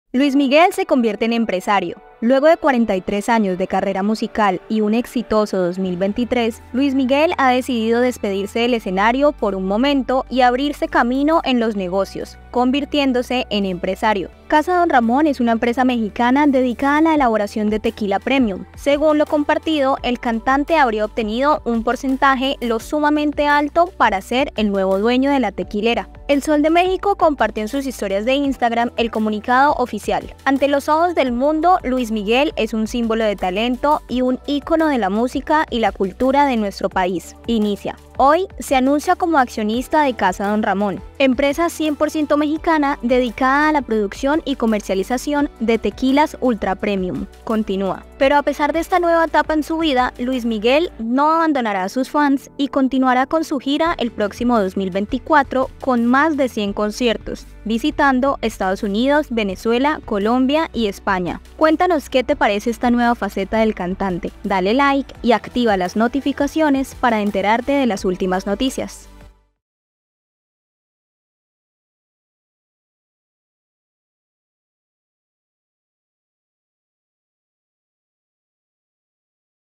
Luis Miguel comparte su nueva faceta como empresario | La Opinión
Luis Miguel rompe con la tradición musical, para embarcarse en un emocionante capítulo empresarial. El ícono de la música latina se convierte en accionista de Casa Don Ramón, una destacada tequilera mexicana. ¡Descubre los detalles de su incursión en el mundo de los negocios y cómo combinará esta nueva faceta con su esperada gira en 2024!